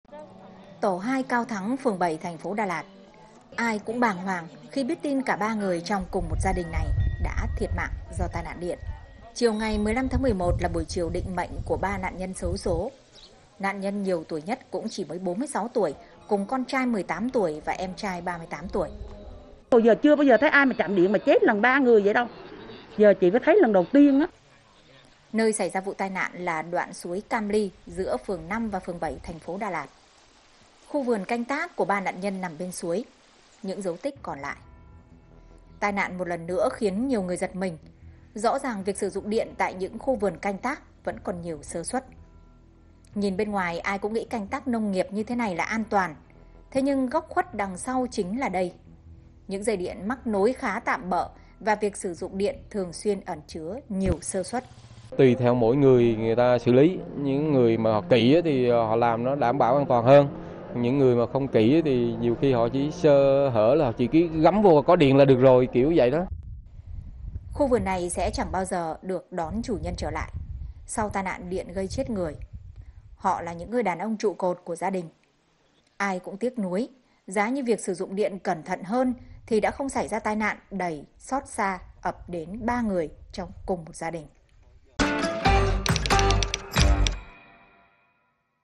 SỬ DỤNG ĐIỆN BẤT CẨN TRONG KHU SẢN XUẤT GÂY CHẾT NGƯỜI | VTV5
Cần phải hết sức cẩn trọng khi sử dụng điện. Hiểm họa tai nạn điện rất dễ xảy ra trong mùa mưa hiện nay tại những khu vườn canh tác ở Lâm Đồng, nơi lâu nay nhiều người vẫn còn nhiều sơ suất khi sử dụng điện.